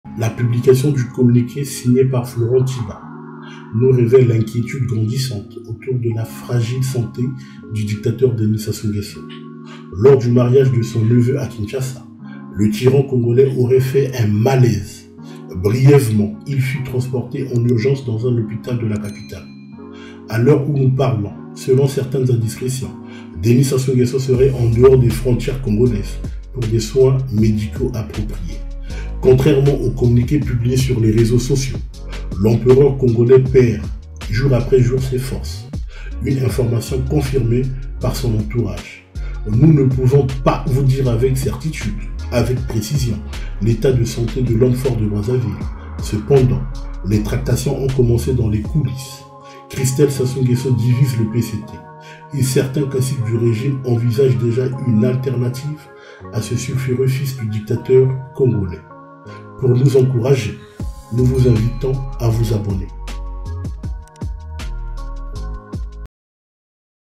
DENIS SASSOU NGUESSO EN DIFFICULTÉ (DÉCRYPTAGE)
Le Dictateur Denis Sassou Nguesso en difficulté selon ses proches.

DEVENIR MEMBRE

NOUS SUIVRE

NOUS CONTACTER

COPYRIGHT

MONTAGE